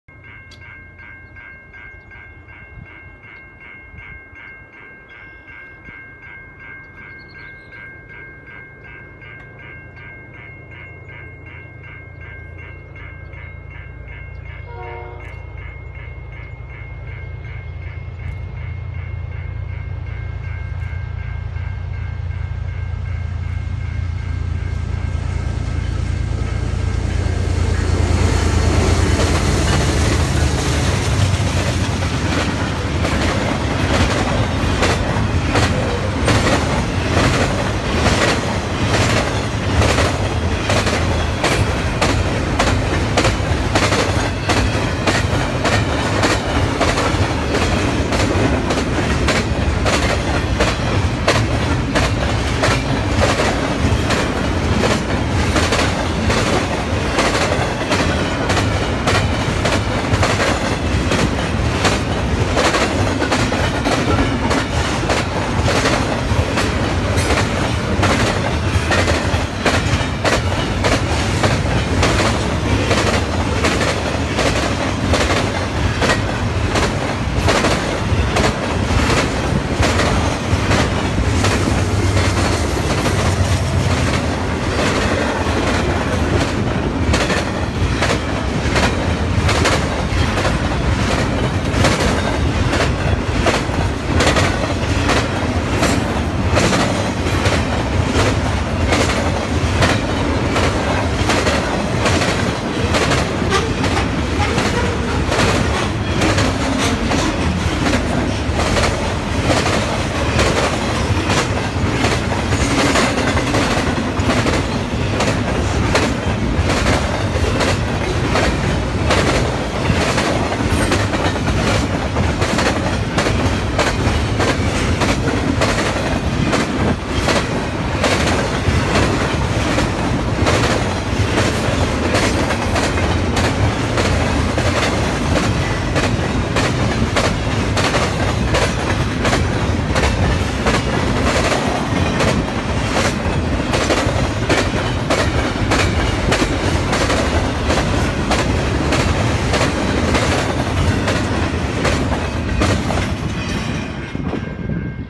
CN 111 at Dugald, Manitoba 2016/05/28
CN 111 rolled through the sleepy town of Dugald on a Saturday morning around 7:30. They had CN 2262 and 2598 on the head end and CN 2528 was assisting as DPU in the middle of the train.